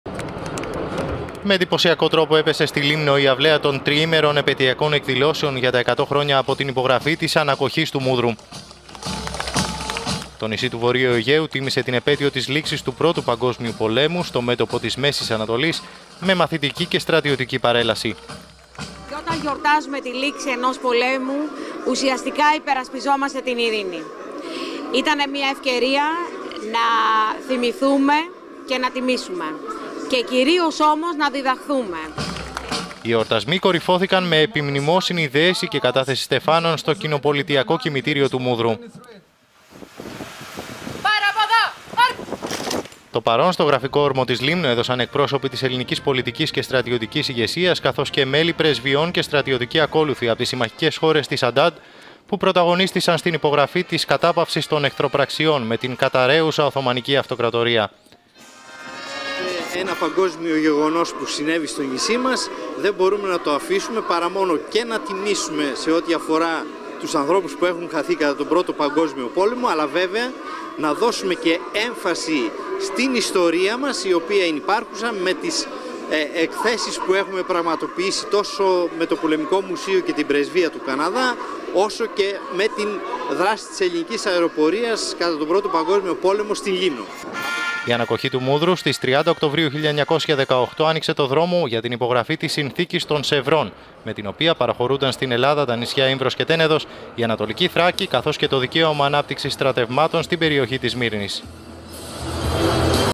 Λήμνος: 100 χρόνια από την υπογραφή της ανακωχής του Μούδρου
Λαμπροί εορτασμοί για τη ιστορική ανακωχή, που έβαλε τέλος στις εχθροπραξίες του Α΄ Παγκοσμίου Πολέμου στο μέτωπο της Μέσης Ανατολής, σηματοδότησε το τέλος της Οθωμανικής Αυτοτατορίας και άνοιξε τον δρόμο για την μεγάλη περιπέτεια του Ελληνισμού …